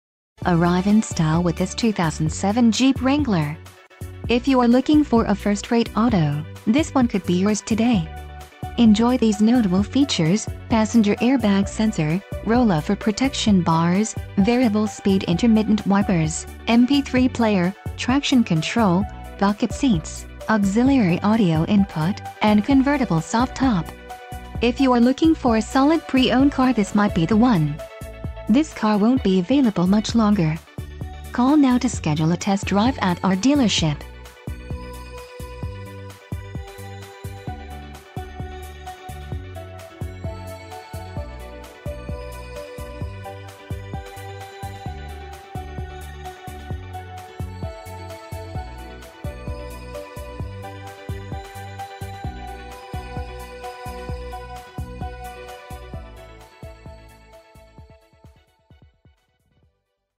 2007 Jeep Wrangler - Benny Boyd Lamesa Chrysler Dodge Ram J
Includes a CARFAX buyback guarantee!! 4 Wheel Drive, never get stuck again** Less than 89k Miles* Climb into this superior Vehicle and experience the kind of driving excitment that keeps you smiling all the way home. Oh yeah! Safety equipment includes: ABS, Traction control, Passenger Airbag, Stability control - With anti-roll control...Relax in the comfort of features like: Convertible roof - Manual, Air conditioning, 202 horsepower, 3.8 liter V6 engine, Tilt steering wheel... a Better buy from Benny Boyd